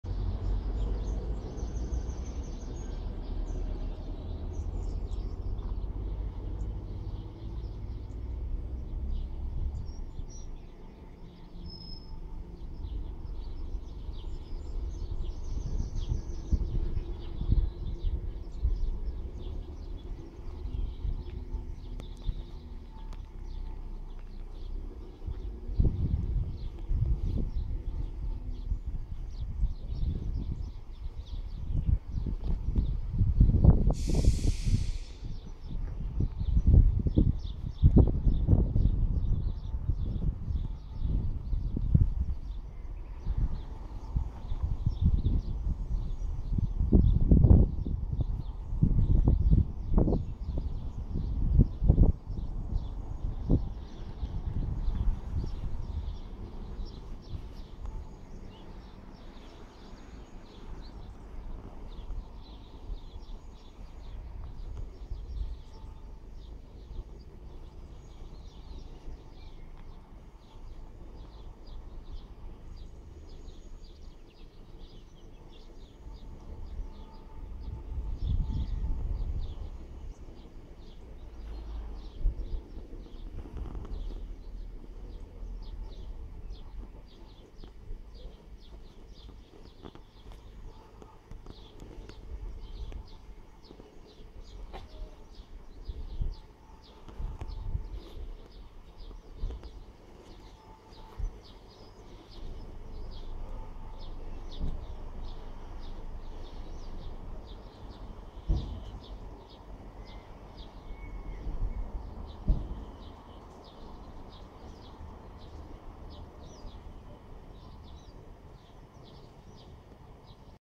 Honiton Train Station
Train Service From London Waterloo To Exeter St Davids Calling At Honiton